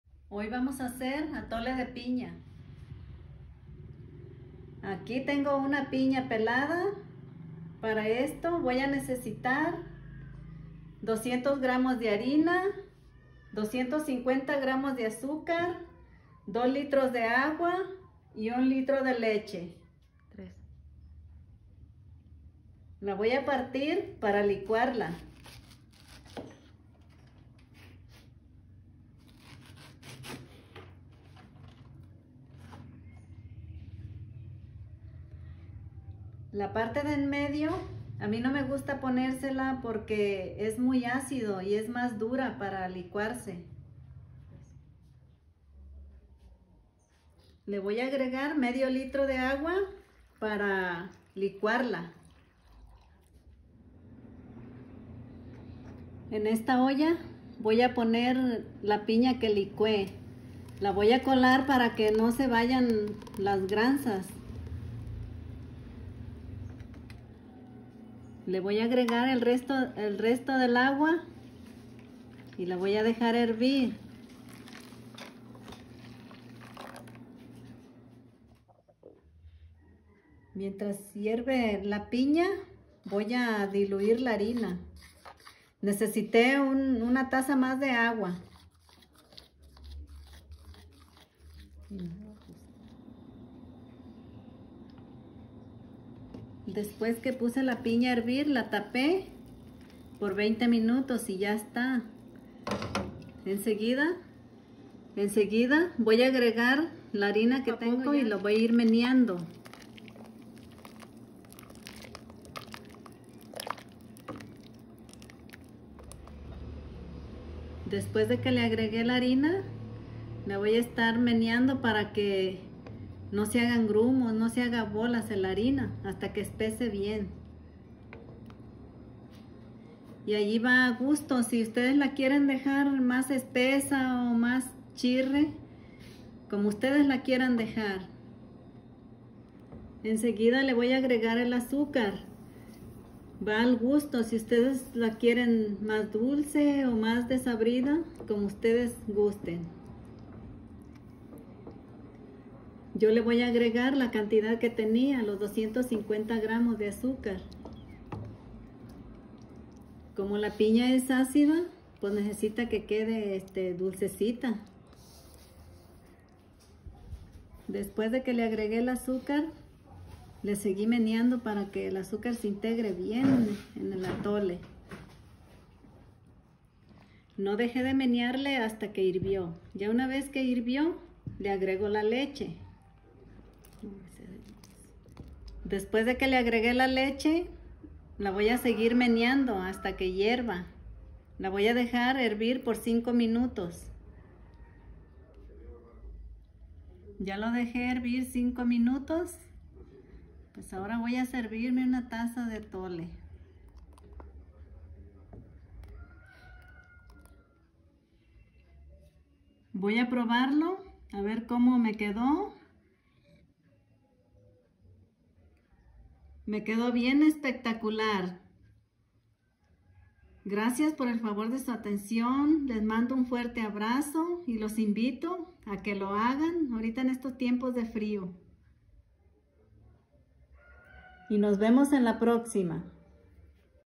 Atole de Pina con Leche no se Corta
Ingredientes:
1 pina
200 grms. de harina de trigo
250 grms. de azucar
1 litro de agua y una taza
1 litro de leche